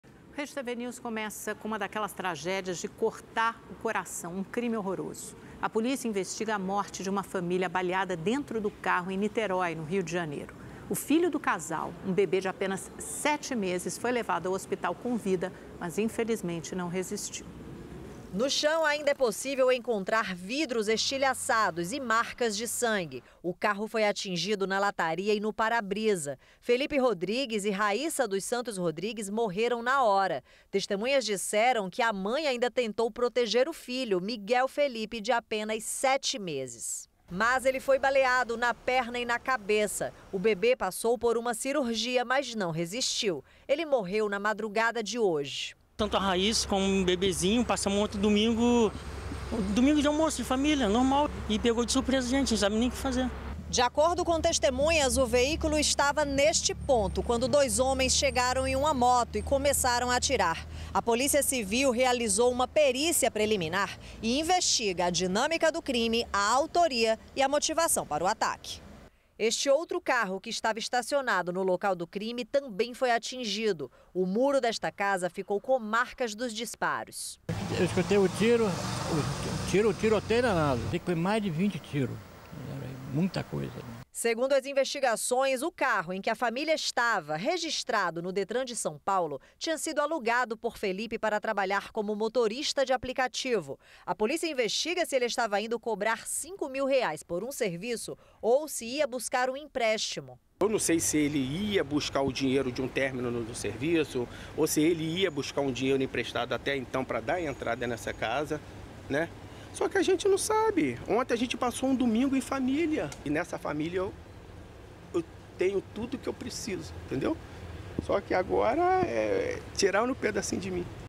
Ataque em Niterói: família é morta à tiros dentro do carro
A polícia do Rio de Janeiro investiga a morte de uma família baleada dentro de um carro em Niterói. O filho do casal, de apenas 7 meses, chegou a ser socorrido, mas não resistiu.

A família voltava para sua casa no Pacheco, em São Gonçalo, após um almoço na casa de familiares, em Santa Izabel, outro bairro da cidade.